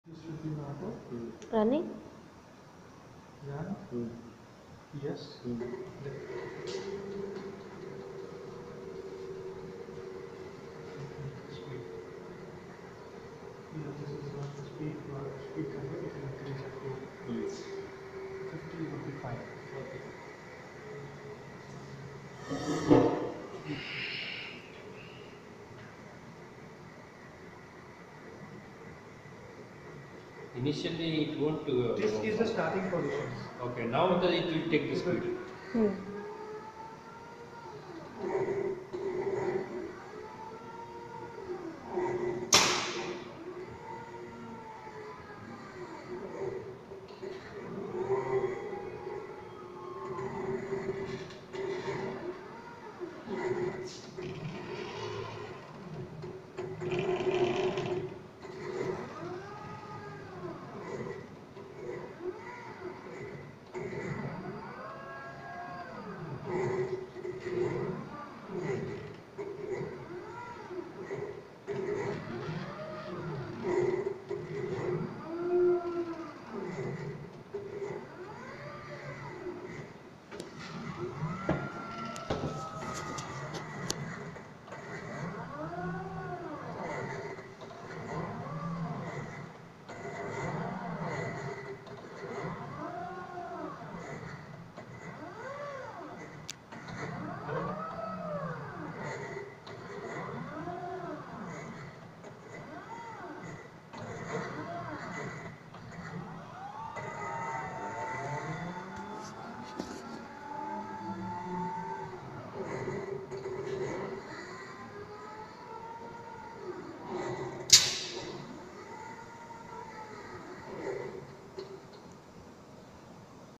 BRABO EDUCATION CELL AT BITS PILANI DUBAI CAMPUS
To make students acquainted with Robotics and Automation, BRABO, India's first Indigenous Articulated Industrial Robot have provided education cell to BITS PILANI DUBAI CAMPUS.